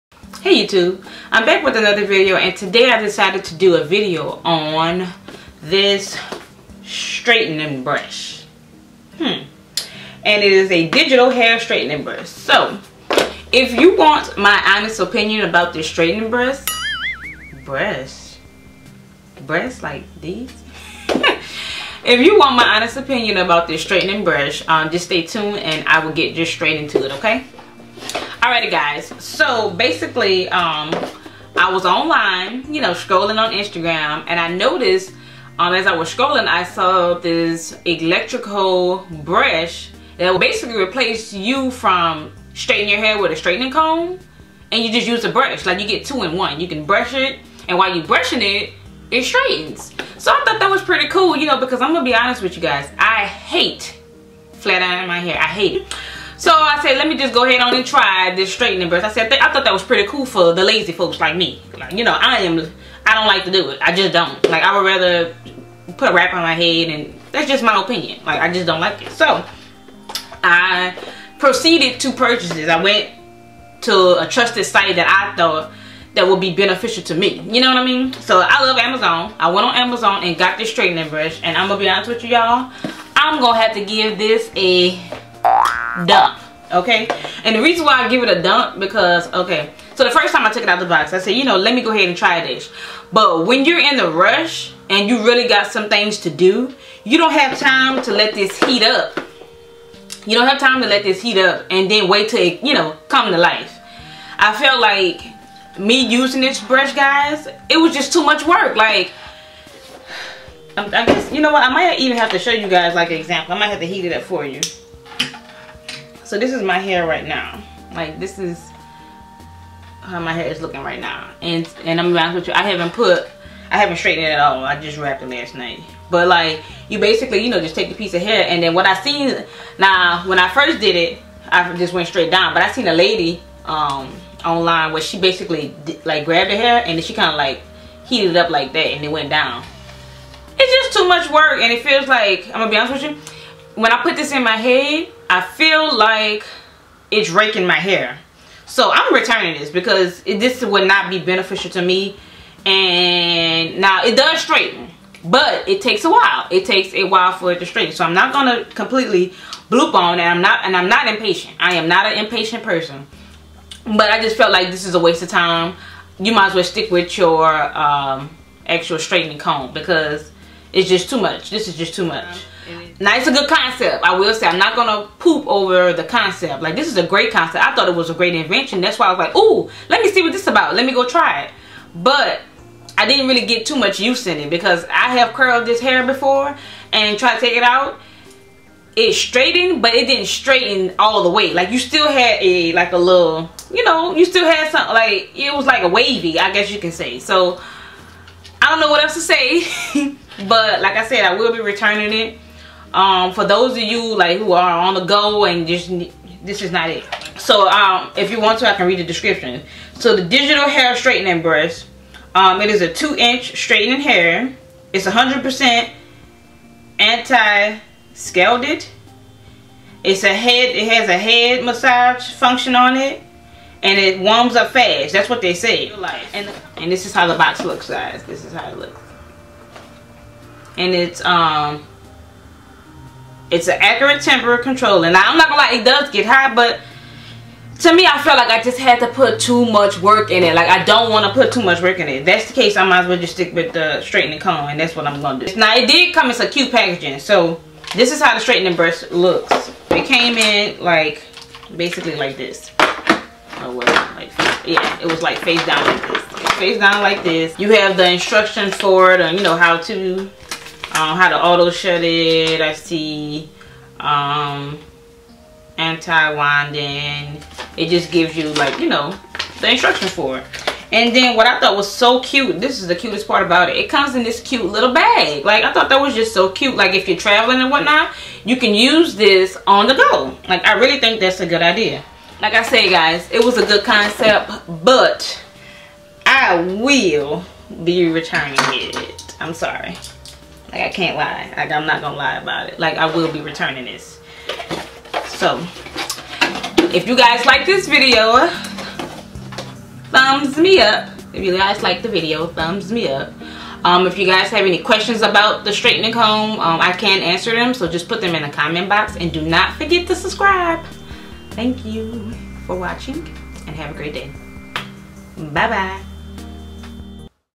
Straightener Brush Review
Fast Brush Hair Straightener review. This video is mainly a review on the new straighten brush that recently came out. I have returned because it was not beneficial to me.